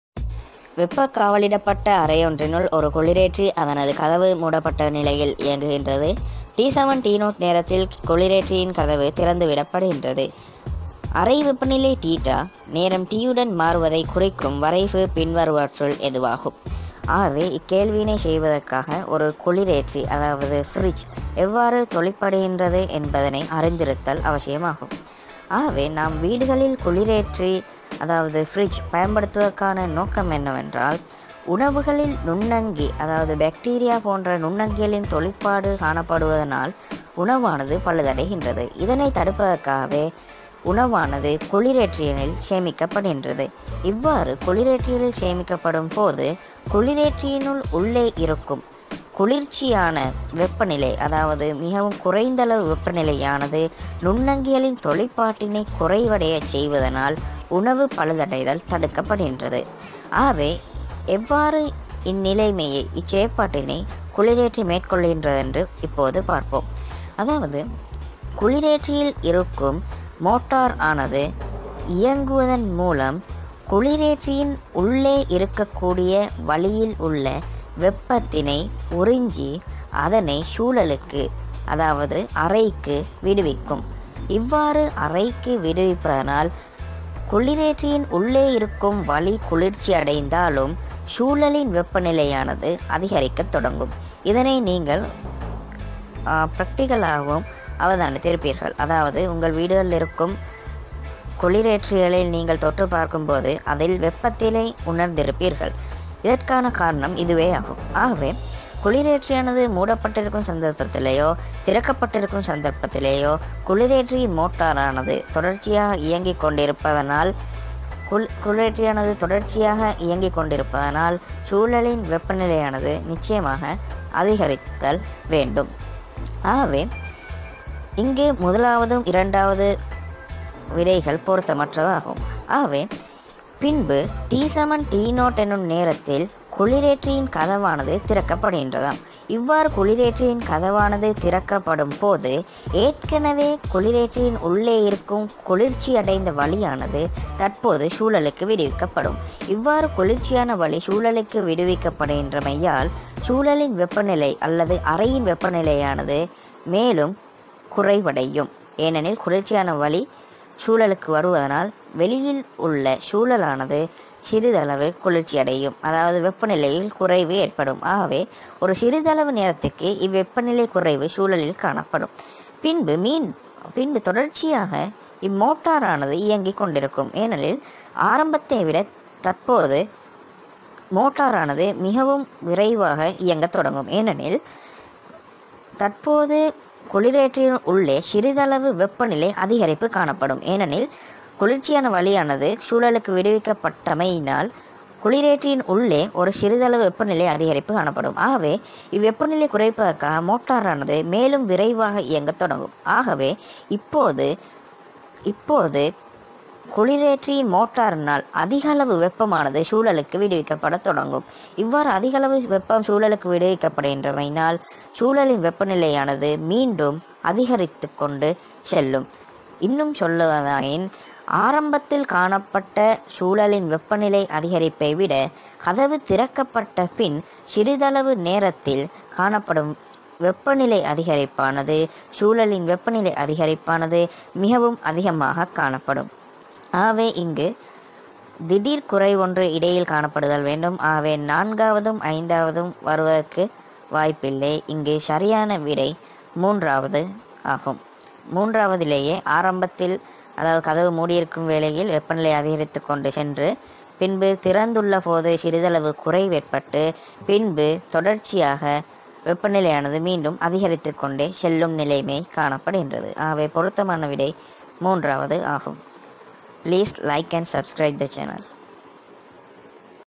A/L PHYSICS PASTPAPER QUESTION IN TAMIL!!குளிரேற்றி(Fridge)இல் நடைபெறும் வெப்பசெயன்முறை|STUDYPHYSICS
Alevel physics pastpaper question explaining in tamil for tamil medium students